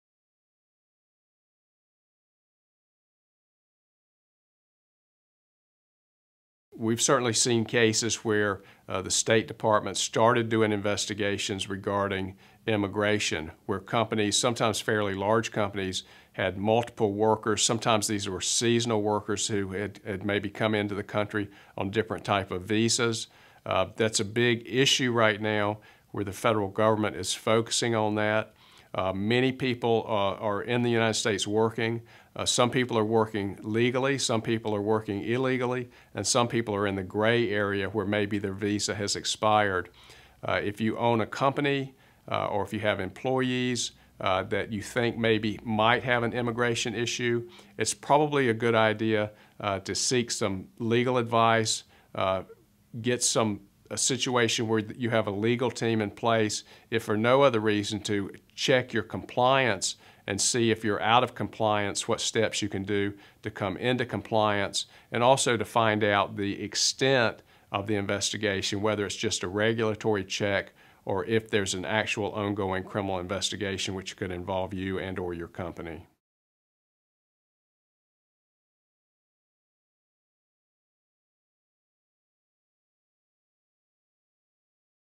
Should I answer questions asked by the state department investigator?
We’ve certainly seen cases where the State Department started doing investigations regarding immigration when companies, sometimes fairly large companies, had multiple workers (sometimes these were seasonal workers) who had maybe come into the country on different types of visas. This is a big issue right now, and the federal government is focusing on that.

Many people are in the United States working – some people are working legally, some people are working illegally, and some people are in the grey area where maybe their visa has expired. If you own a company or if you have employees who you think might have an immigration issue, it’s probably a good idea to seek some legal advice or get a legal team in place, if for no other reason than to check your compliance and see if you’re out of compliance and what steps you can do to come into compliance. Also, to find out the extent of the investigation, whether it’s just a regulatory check or if there’s an actual ongoing criminal investigation which could involve you and/or your company.

1012 38th Ave. N.
Myrtle Beach, SC
United States